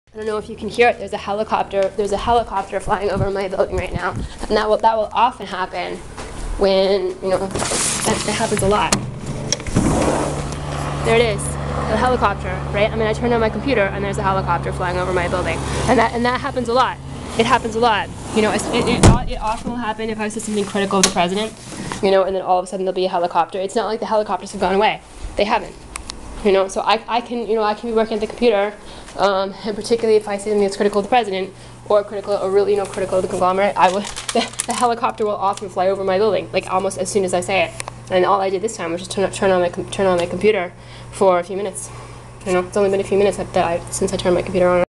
10/15/13: Video from 10/15/13: Helicopter flying over my building
Copyright L. Kochman, October 15, 2013 @ 9:02 p.m.